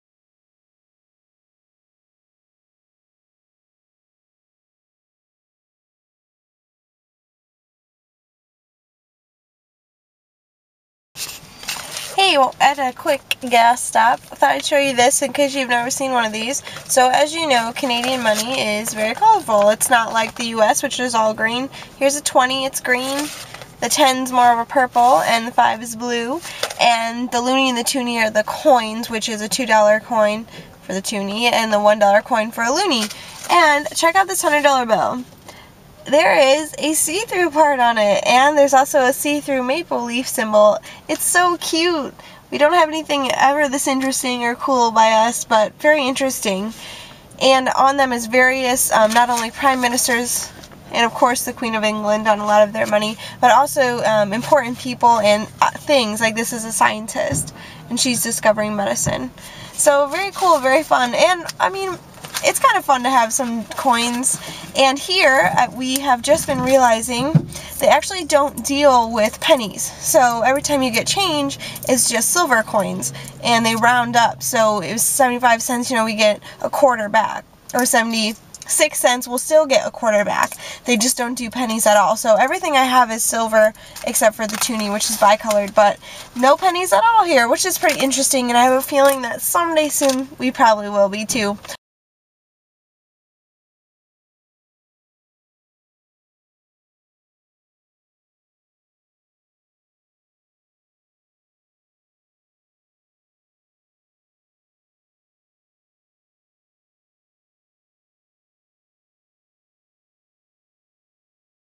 It is off to Canada!!!
Join me on trip to Canada. First stop: right over the border to Windsor to see Chelsea Handler at Ceasers, then to Ottawa, then Montreal, then QC, then Prince Edward County!

This short little video is about the beautiful money Canada has and a fun fact like milk comes in bags! :)

Love, Cortnie